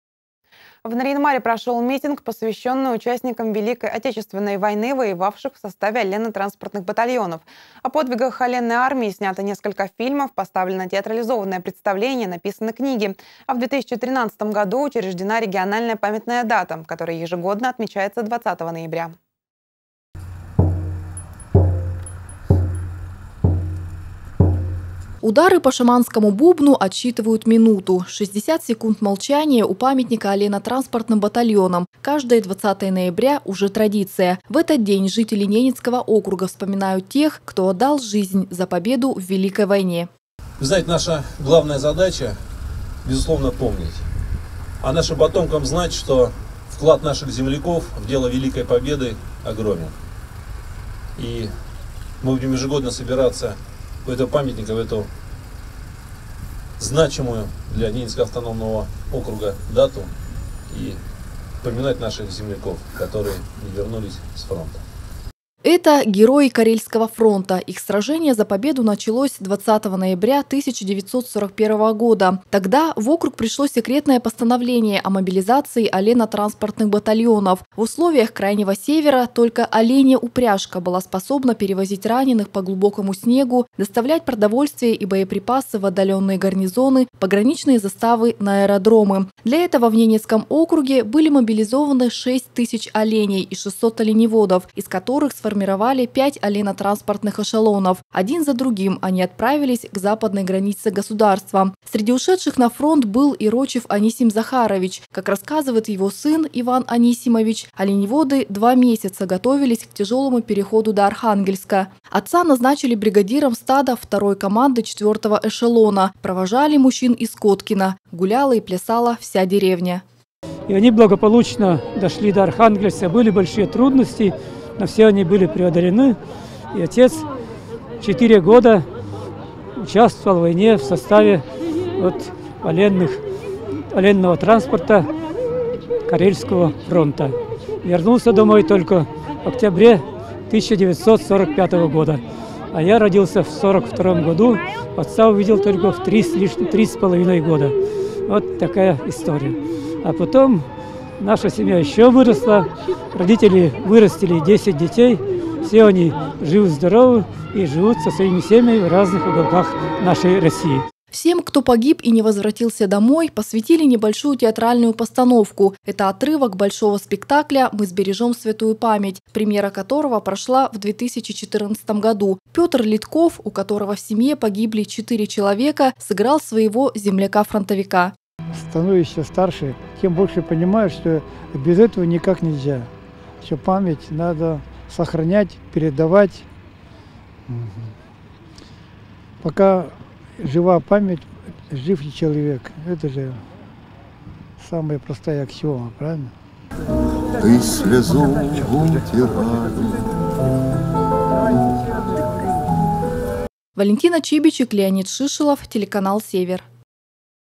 В НАО прошел митинг, посвященный Дню памяти оленно-транспортных батальонов
В Нарьян-Маре прошел митинг, посвященный участникам Великой Отечественной войны, воевавших в составе оленно-транспортных батальонов. О подвигах оленной армии сняты несколько фильмов, поставлено театрализованное представление, написаны книги. А в 2013 году учреждена региональная памятная дата, которая ежегодно отмечается 20 ноября.

Удары по шаманскому бубну отсчитывают минуту. 60 секунд молчания у памятника оленно-транспортным батальонам каждое 20 ноября уже традиция. В этот день жители Ненецкого округа вспоминают тех, кто отдал жизнь за победу в Великой войне.